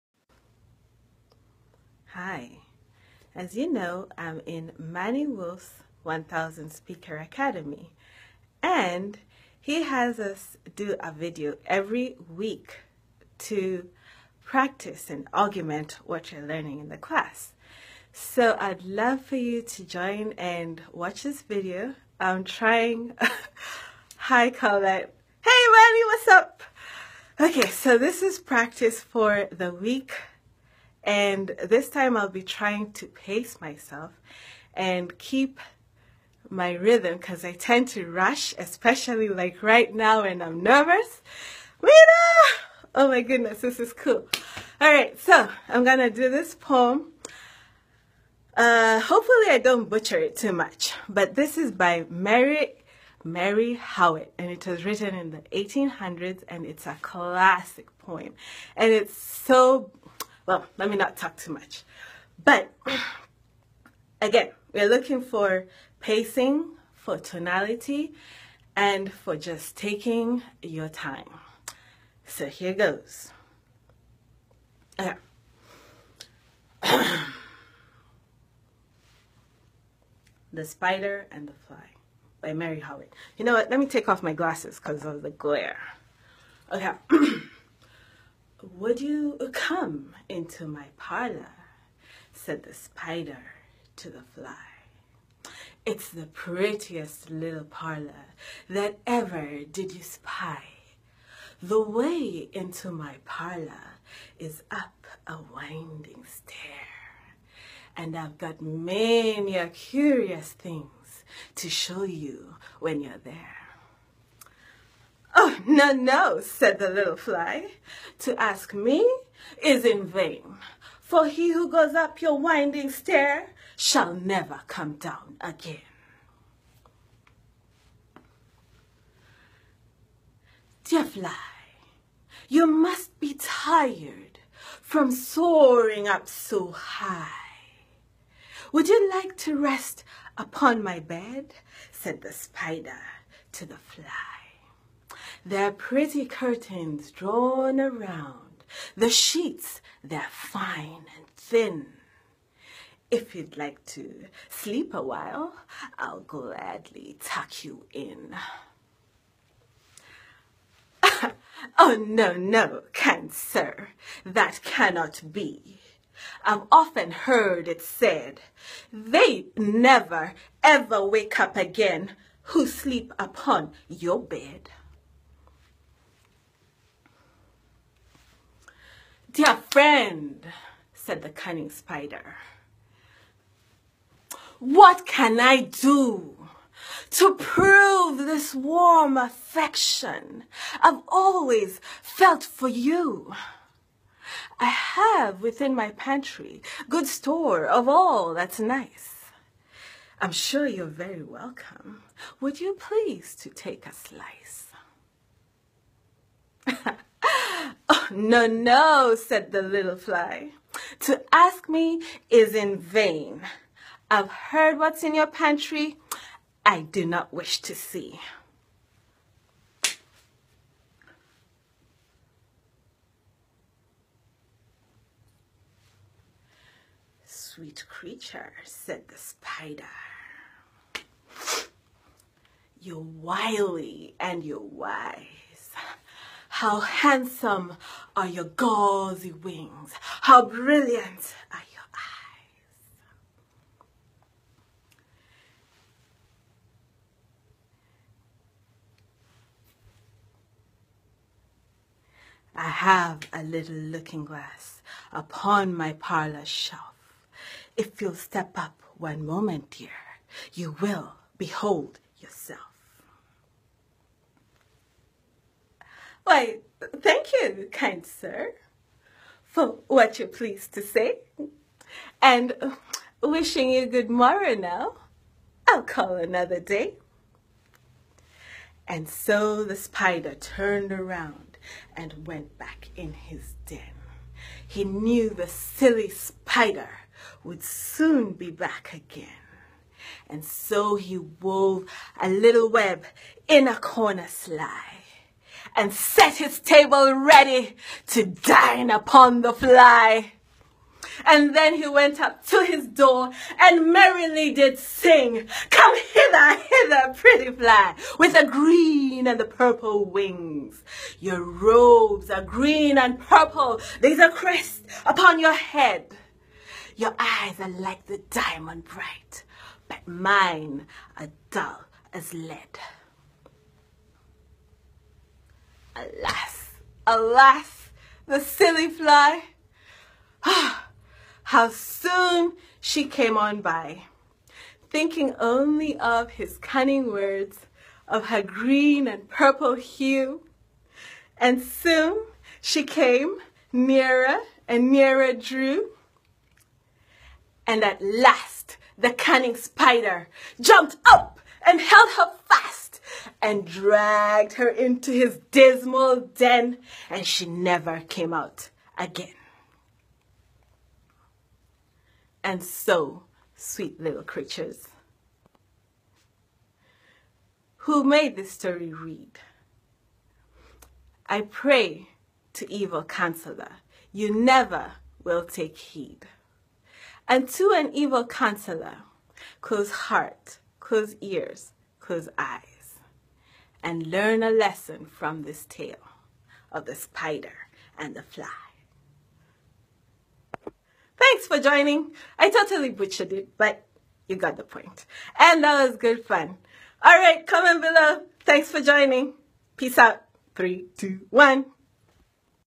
The Epic Battle of the Spider and the Fly
listen to the ONLY Fascinating "Short STORY"/poem I still remember it from being a kid (centuries ago,haha) a tale of Seduction, Cunning, treachery, and death.
Let's see how much Tonality, BodyLanguage StoryTelling we can pack in here, lolz. 1000Speaker Academy

Feel free to critique at will, I already know the recitation of words wasn't 100% but the whole purpose is to practice public speaking. You get the idea.